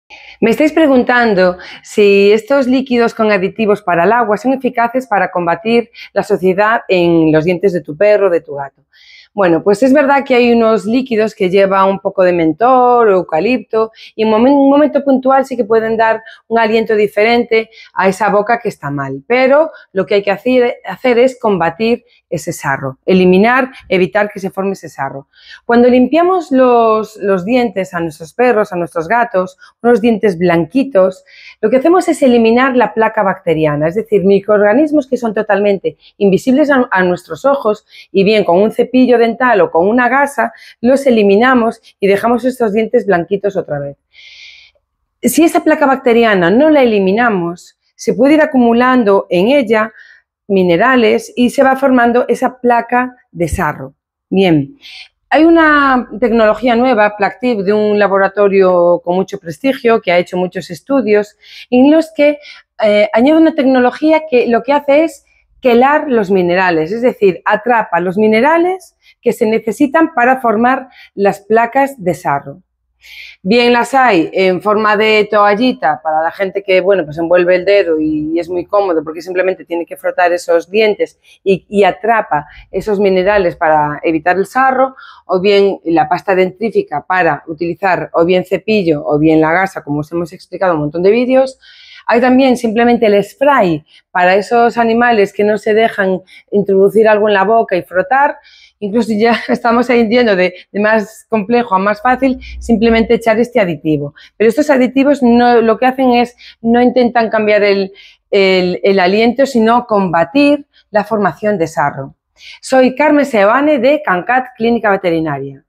Son eficaces los aditivos para el agua de bebida para evitar el sarro?
Es indipensable mantener los dientes de tu animal de compañía limpios, por su salud y por la de tu familia.
En este vídeo nuestra veterinaria, Carme Seoane Paredes, explica diferentes productos para ayudarte en la salud bucodental de tu perro o gato.